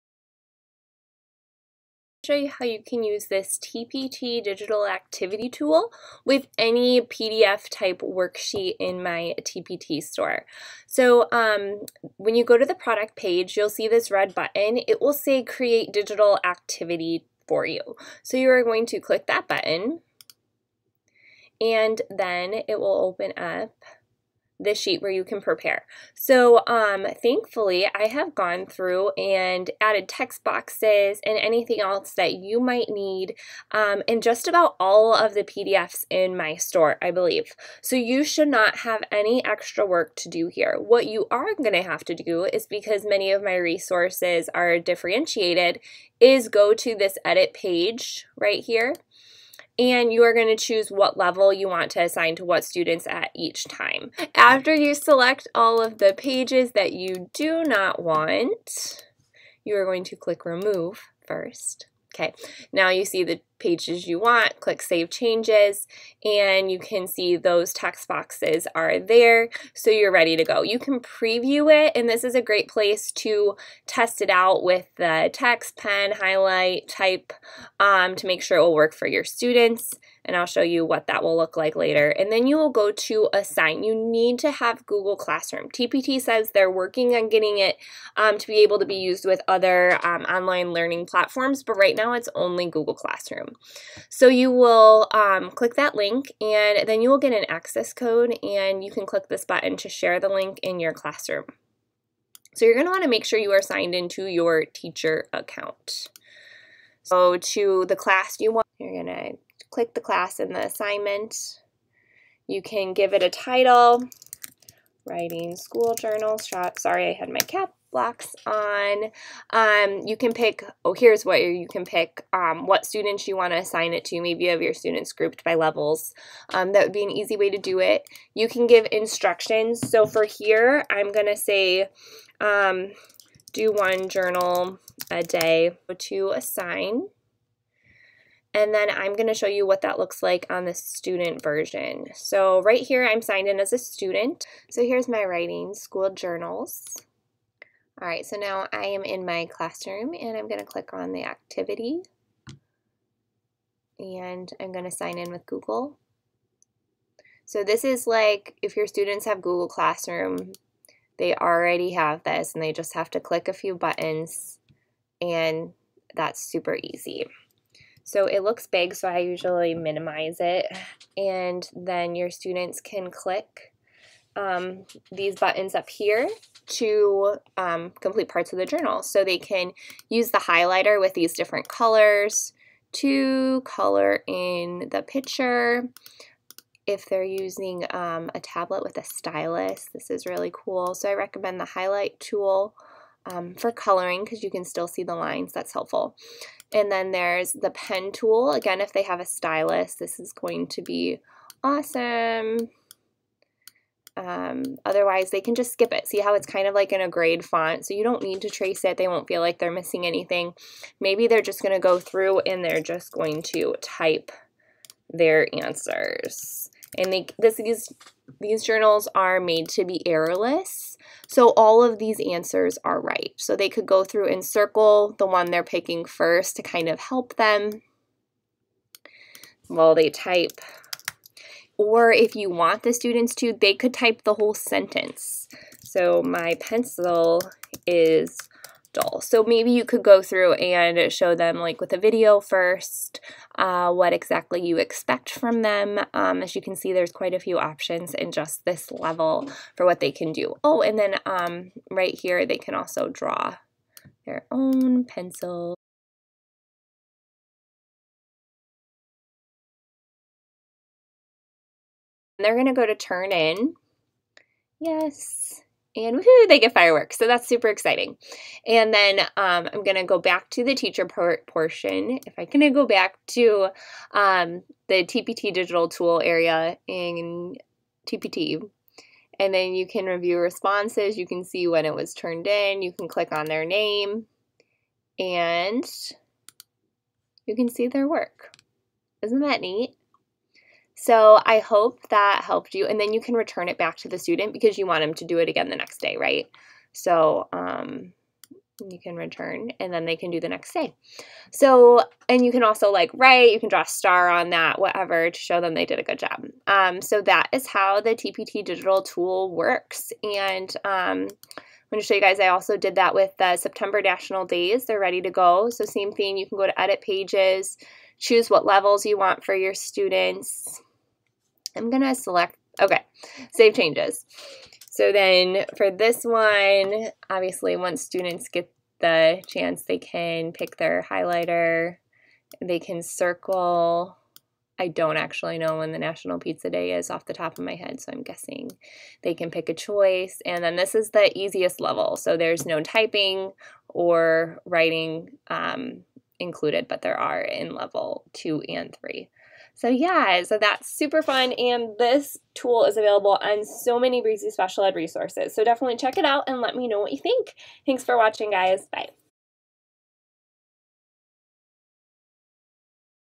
Use the TpT Digital Tool with Google Classroom
Breezy Special Ed worksheets and packets are available with the TpT digital tool! Check out how in this video tutorial. For a written tutorial and list of resources available to use with this tool, visit this blog post